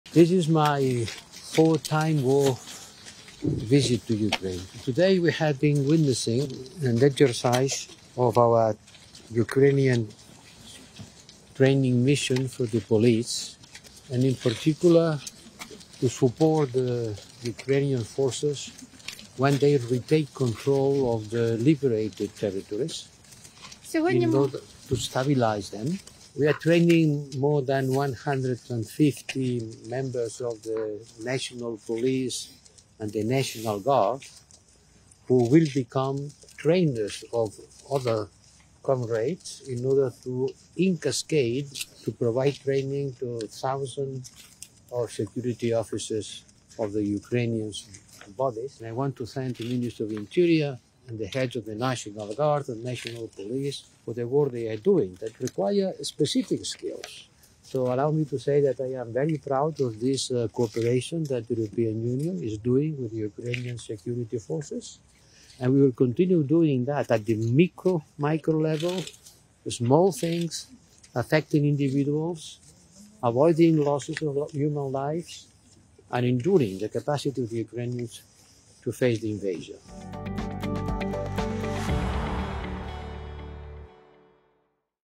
HR/VP Josep Borrell in Ukraine | Joint Press conference | 06/02/2024
HR/VP @JosepBorrellF visited today a @EUAM_Ukraine training for the Ukrainian police, aimed at the stabilisation of the liberated territories. EU trainings will contribute to allow for the return to normal life after de-occupation.